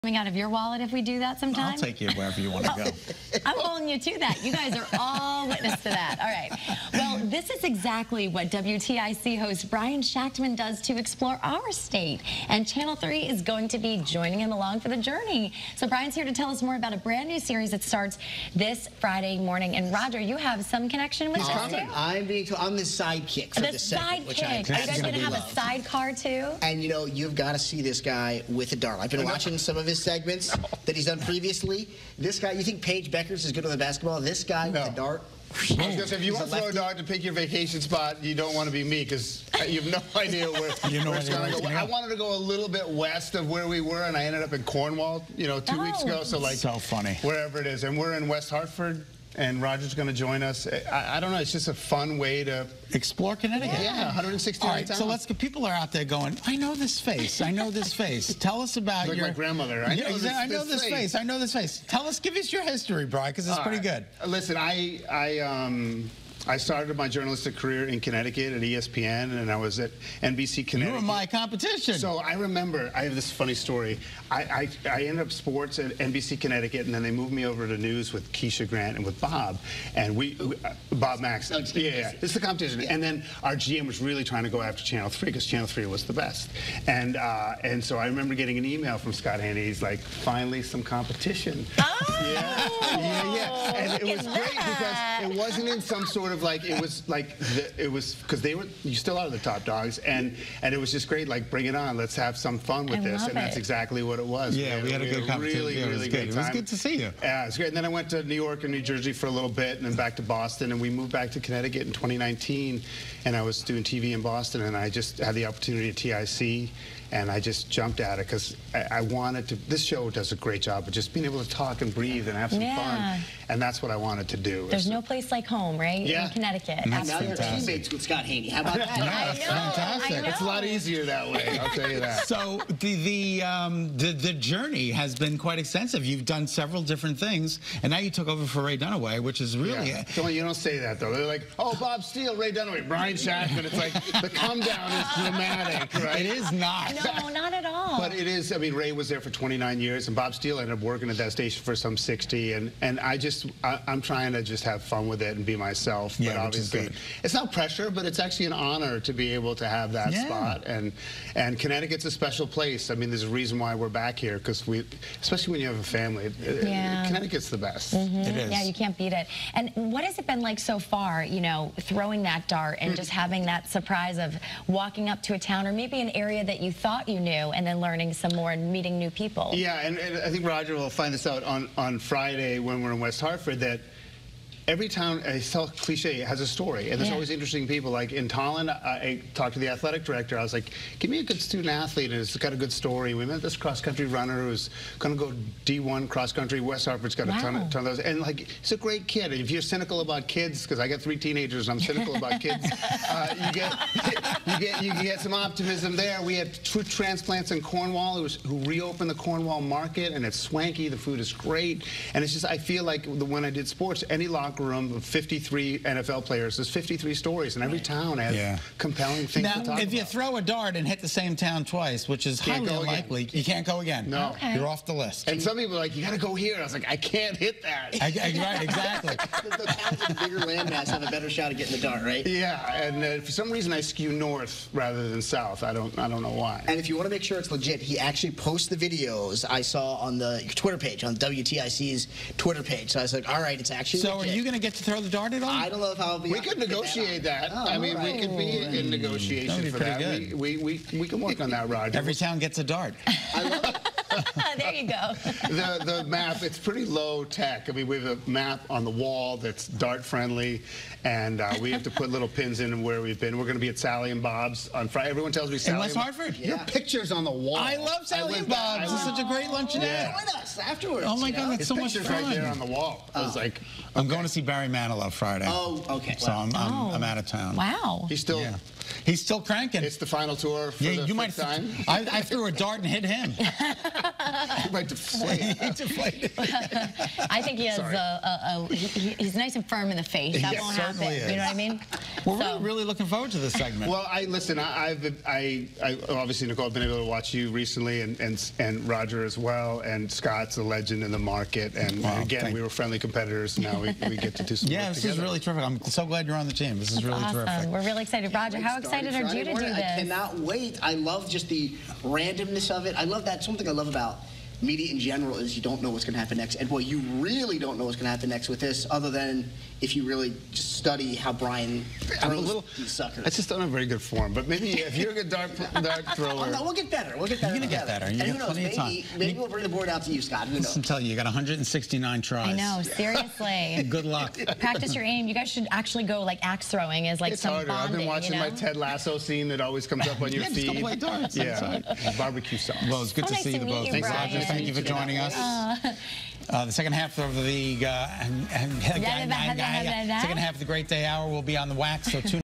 Darting Across CT
Brian Shactman is here to talk about a new series that starts this Friday morning!